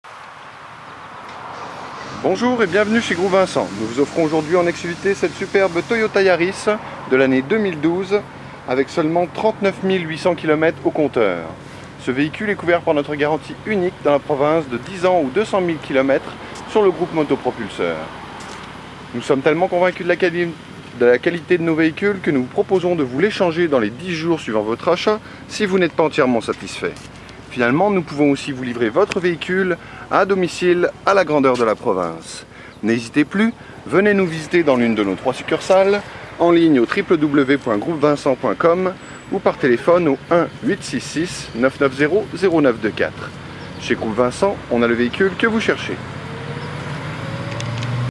TOYOTA YARIS 3-DOOR 2012 (STOCK m0471a)
NE MANQUEZ PLUS VOS APPELS AVEC LE BLUETOOTH

PRÊT POUR L'AVENTURE AVEC SON ATTELAGE

LES COUSSINS GONFLABLES CONTRIBUENT À VOTRE SÉCURITÉ